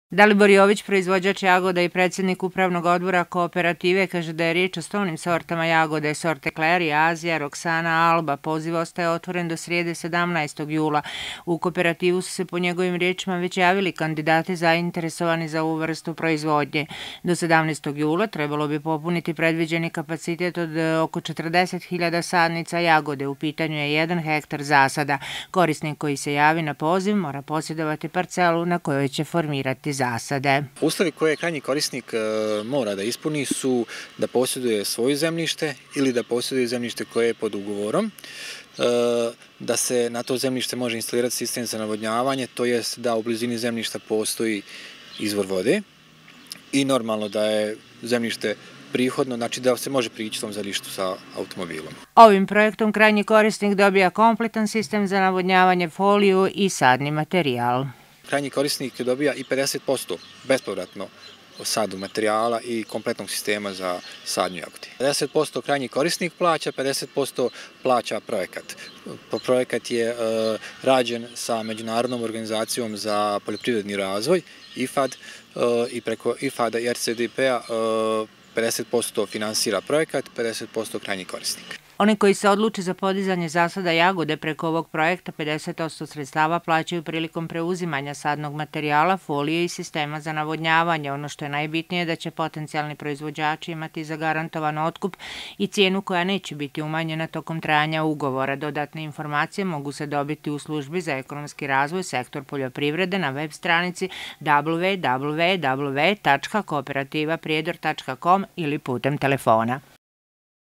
JAVNI POZIV ZA PODIZANJE ZASADA JAGODE
Poljoprivredna zadruga „Kooperativa“ u saradnji s IFAD-om, Međunarodnim fondom za poljoprivredni razvoj, raspisala je javni poziv za odabir korisnika za podizanje zasada jagode.